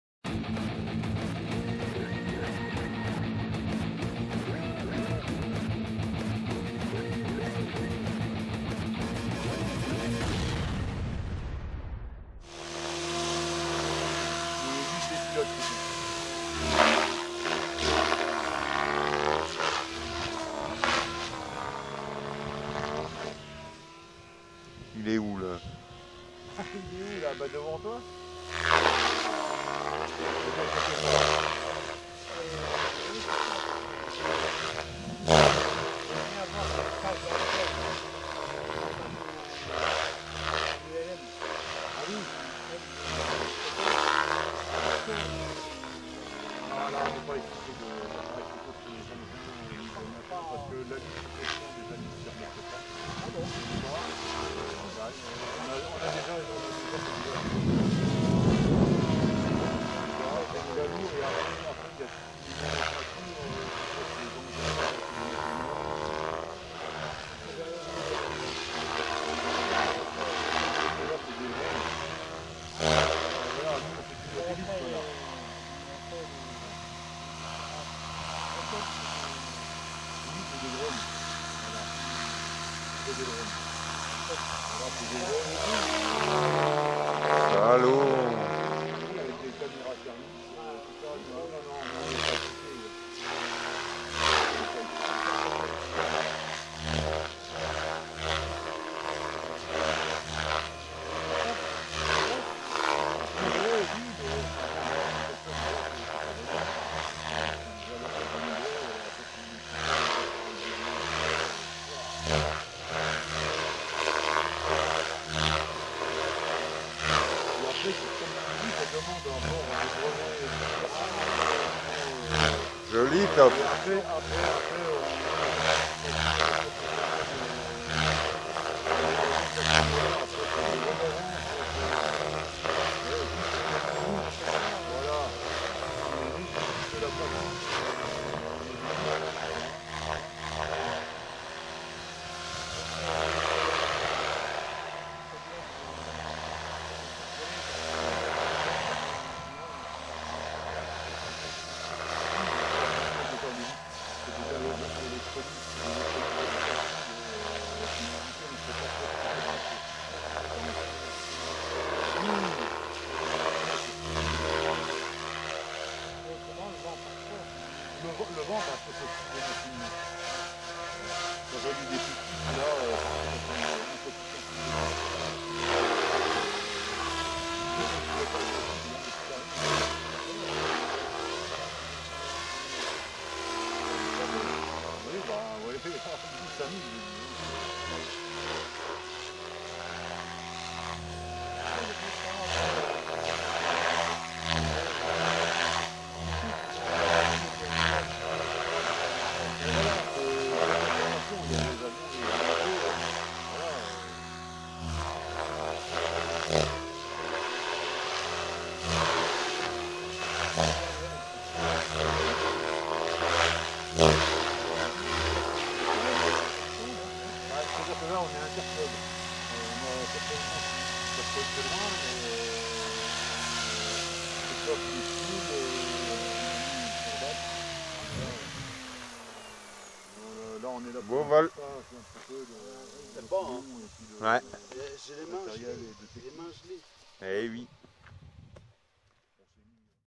Goblin 570 12s Neuron "Tof" P-A-M le 08/10/2016 (Ultra-HD)
Pilote: Tof
Caméra: Manu
Lieu: PAM, 54 Lorraine